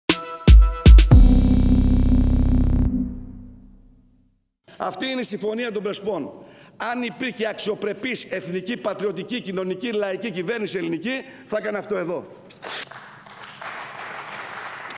Ομιλία του Κυριάκου Βελόπουλου στη Βουλή
Ο Κυριάκος Βελόπουλος έθεσε το ερώτημα τι συζήτησαν στο τετ α τετ που είχαν Μητσοτάκης και Ερντογάν, κάνοντας λόγο για «μυστική διπλωματία». Παράλληλα, κατηγόρησε τα κυβερνητικά στελέχη λέγοντας «είστε φοβικοί απέναντι στην Τουρκία».
Ο πρόεδρος της Ελληνικής Λύσης, μιλώντας για την Συμφωνία των Πρεσπών, είπε ότι αν κυβερνήσει το κόμμα του «θα την σκίσει».
Σχολιάζοντας την επίσκεψη του Αλβανού πρωθυπουργού στην Αθήνα, ισχυρίστηκε «ο Έντι Ράμα παρομοίασε τους Αλβανούς οικονομικούς μετανάστες με την ελληνική εθνική μειονότητα στην Βόρειο Ήπειρο», σημειώνοντας ότι «ο κ. Μητσοτάκης είπε ψέματα ότι δεν μπορούσε να απαγορεύσει στον Ράμα να κάνει εκδήλωση». ΑΠΕ-ΜΠΕ/ΕΡΤ